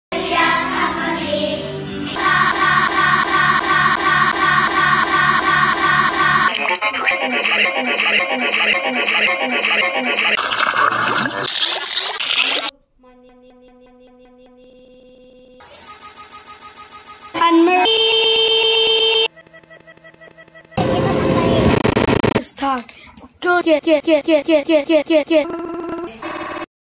YTP - We could have another one
This is my another attempt to make a YTP in PowerDirector. Used some Filters and Effects.